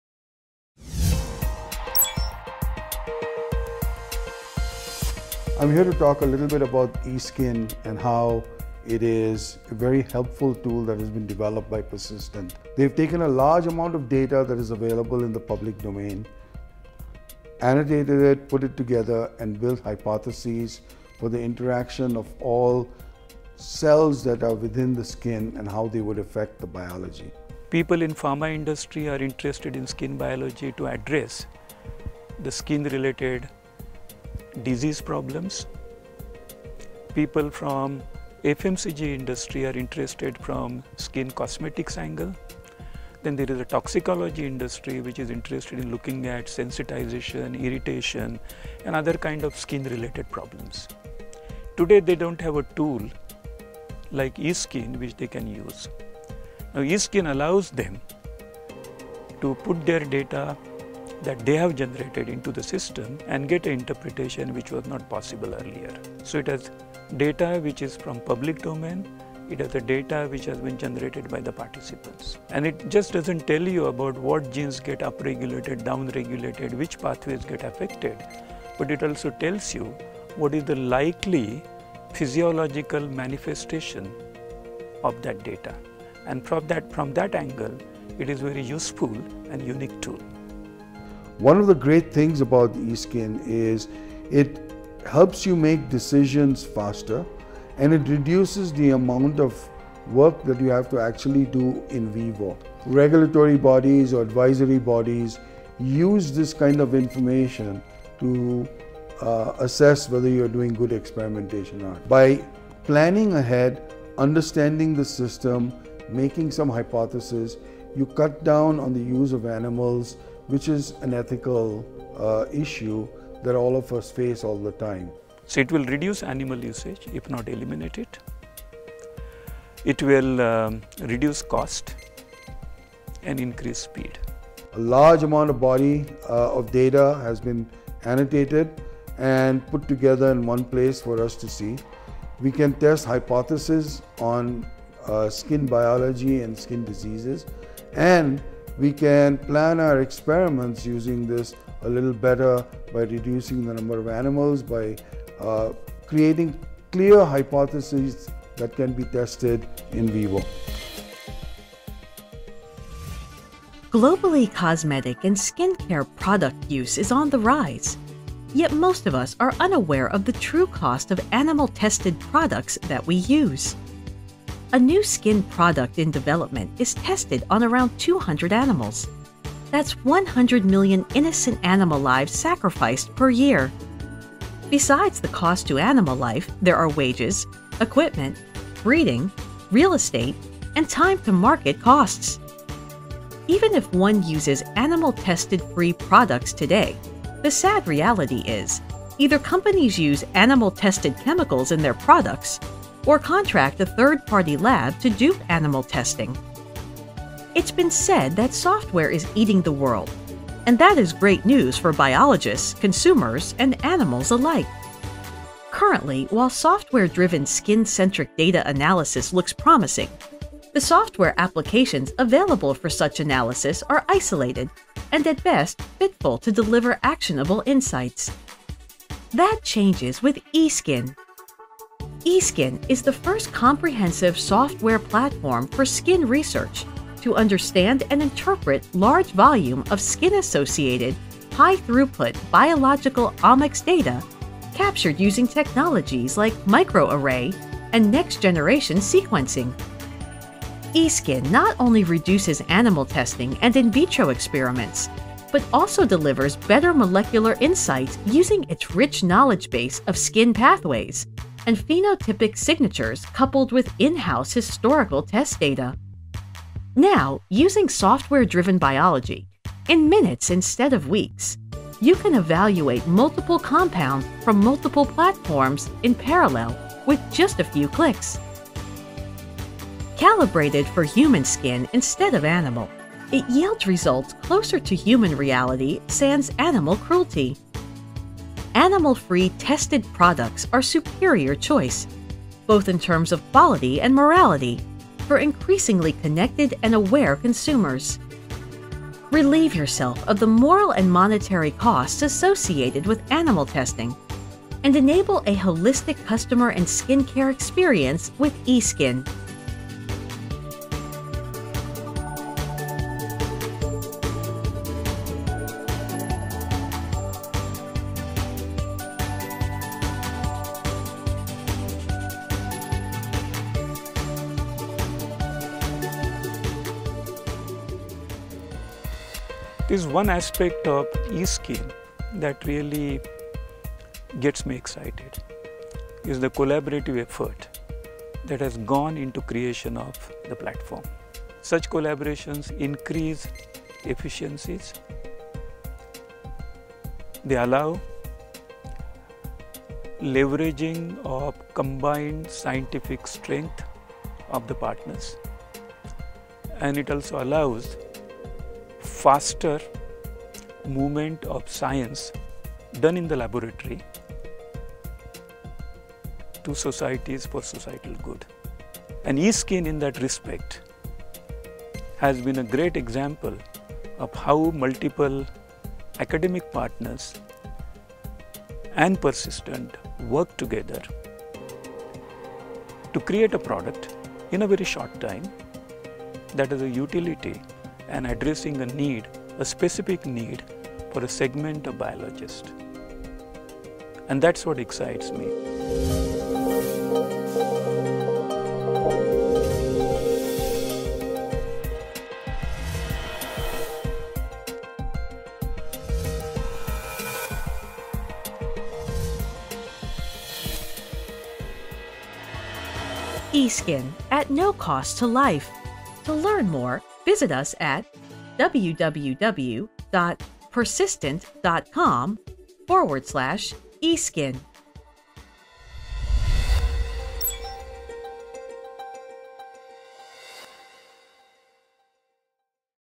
eSkIN: A computational platform for Skin Research
Watch how eSkIN, a computational platform for Skin Research, can revolutionize skin centric analysis. Developed by Persistent Systems and CSIR-IGIB in collaboration with NCL, NII and JNU, the product helps in analyzing high throughput omics data in a time and cost efficient manner.